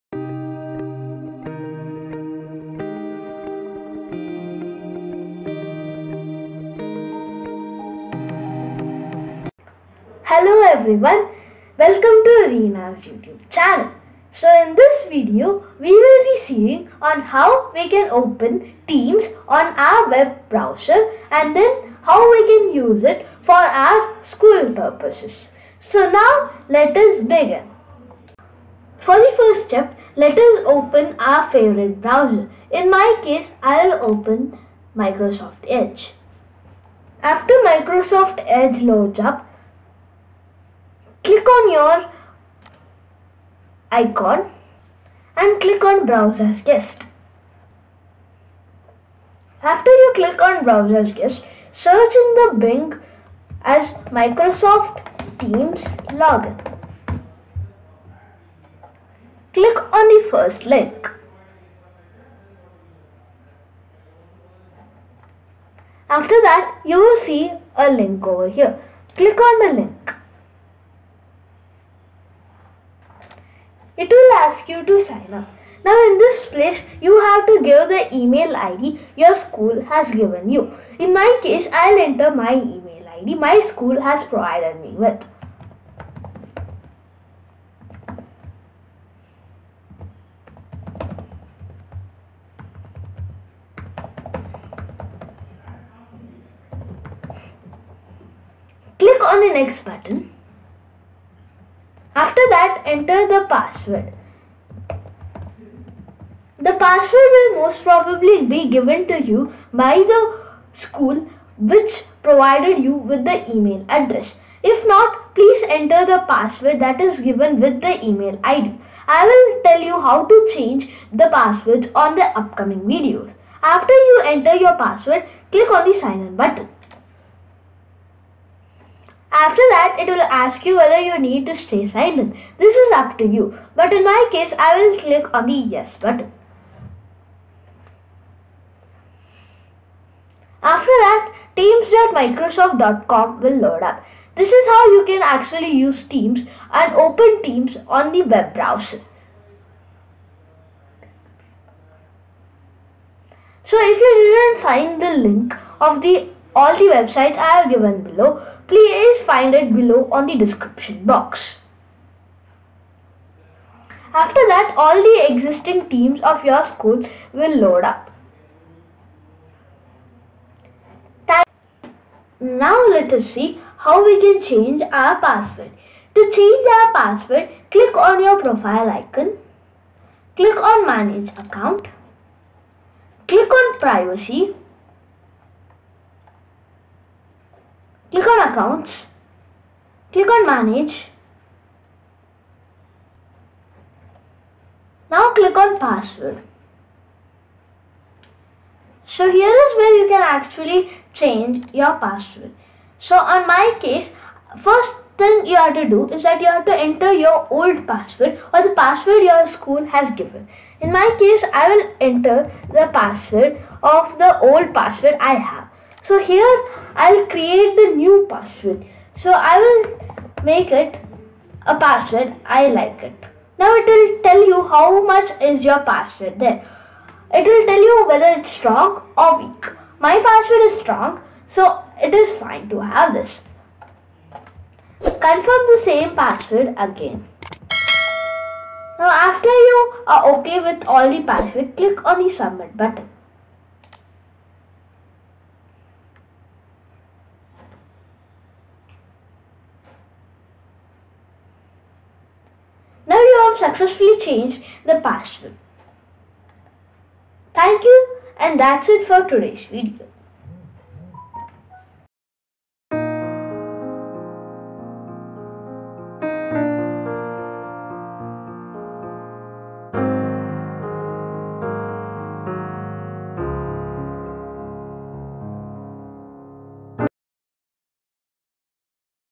Microsoft Teams for school students | How to sign in to Microsoft Teams account | Part 1
Hello Everyone!

In this video, you will learn about how to use the Microsoft Teams web client, log in to teams and much more!

Also enable the notifications so that you dont miss a single update

If you have any doubts, you can always ask it on the comment box below

FAQs

1) What is teams?
Ans. Teams is a application that enables us to connect and interact together. Teams can also be used to share files and is an efficient way to work with your organisation/school.

2) What is the difference between the web client and the desktop app?
Ans. The Web Client can be accessed through the broswer without any installation whereas the desktop app must be installed before using

3)What are the advantages of teams for a school?
Ans. Teams can be used for managing a school in a much more efficient way. it helps the students in interacting with the teachers and have a much more effective in managing the school remotely.

Additional links